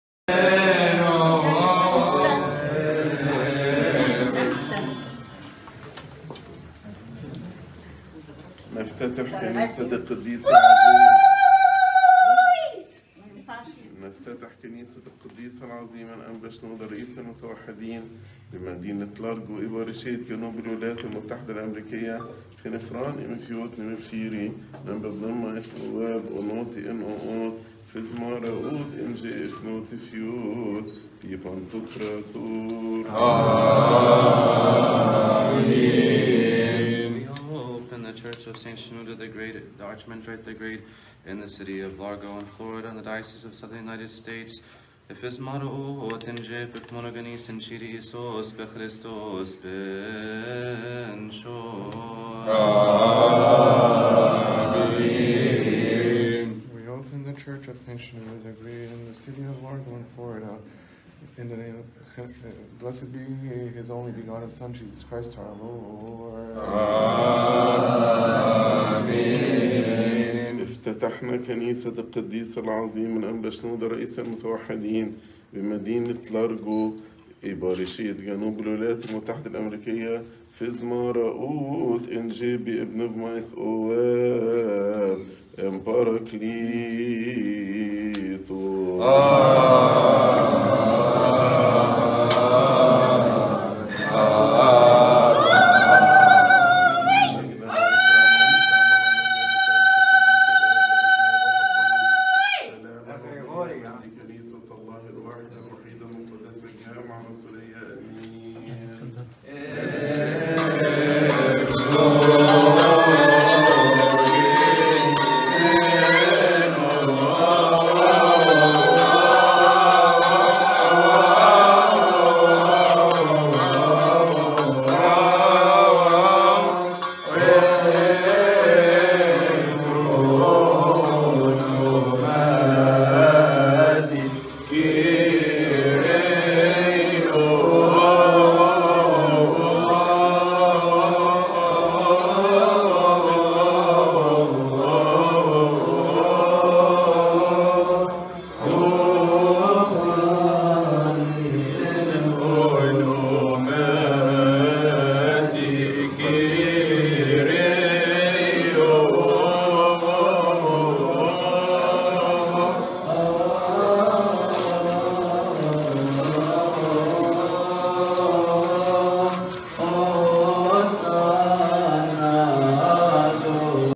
Opening St Shenouda the Archimandrite COC by Bishop Youssef, Bishop Basil &Bishop Gregory~02/28/2022
Opening St. Shenouda the Archimandrite COC with HG Bishop Youssef, HG Bishop Basil & HG Bishop Gregory
St. Shenouda the Archimandrite Coptic Orthodox Church
Largo, FL